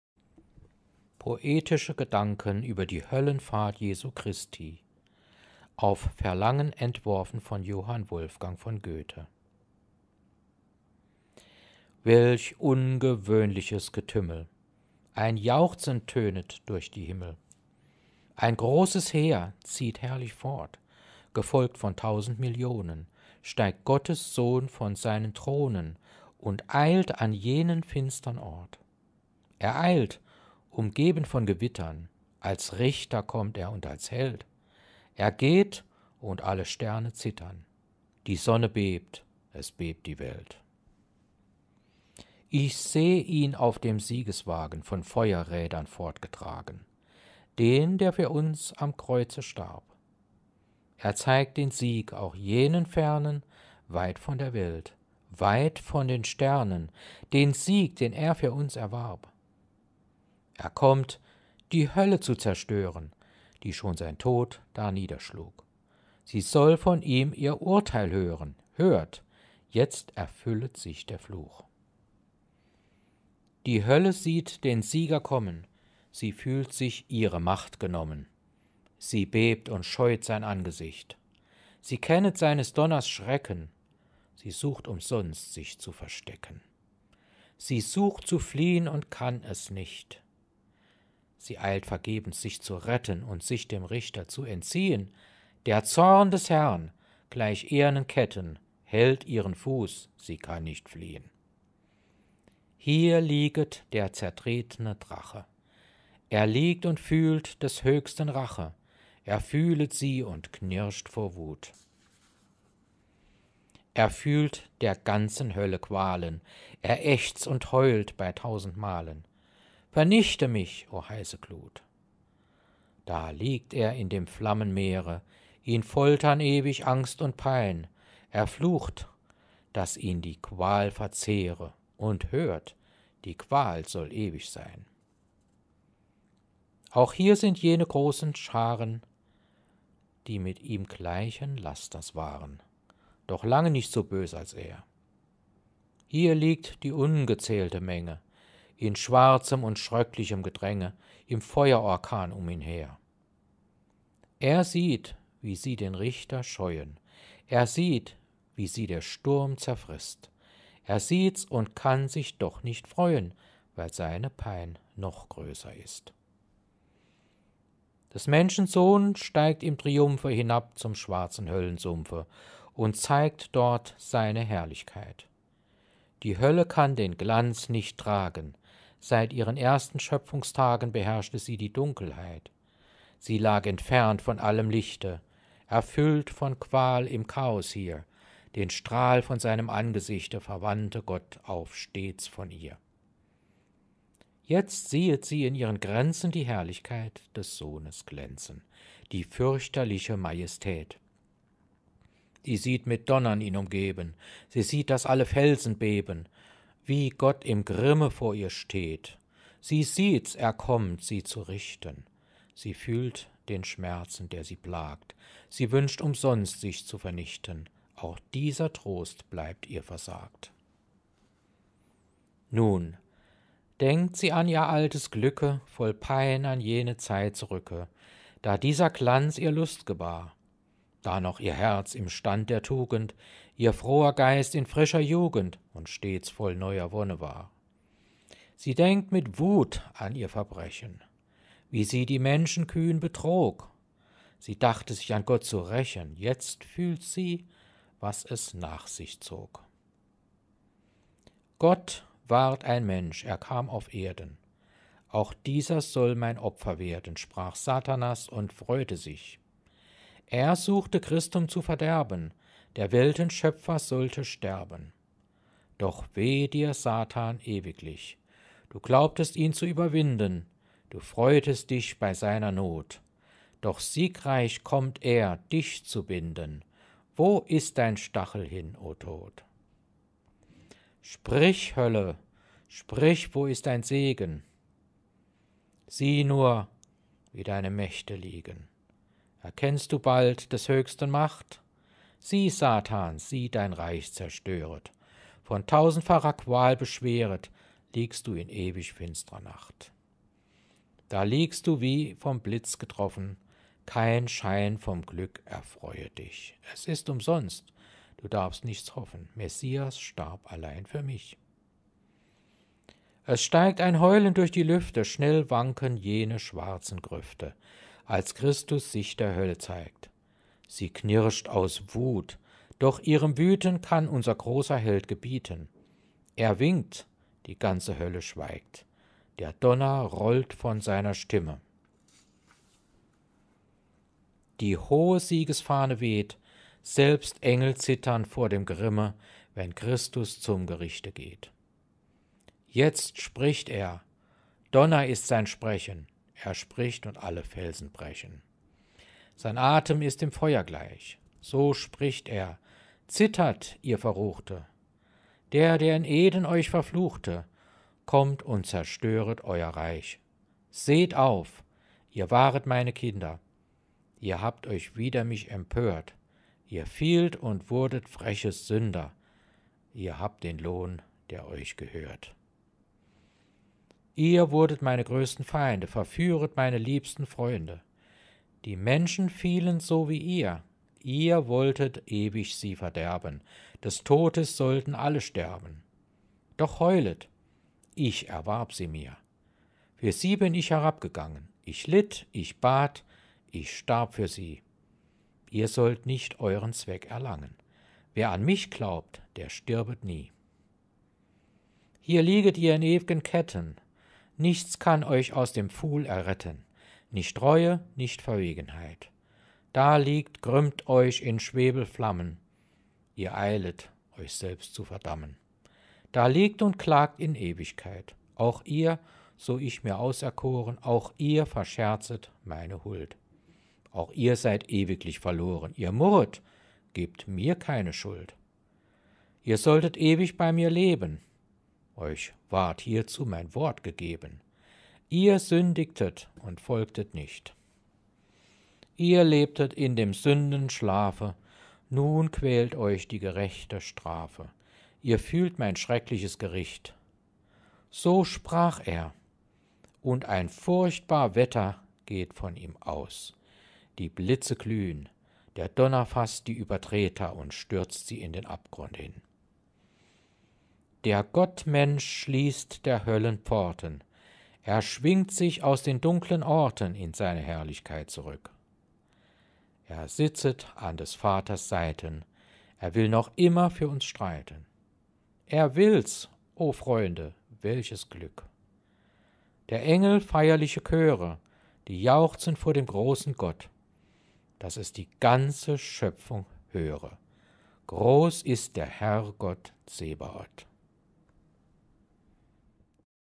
Poetische Gedanken über die Höllenfahrt Jesu Christi
Gelesen von Peter Fritz Walter

Goethe war gerade einmal 15-Jahre alt, als er dieses ziemlich außerordentliche Gedicht schrieb.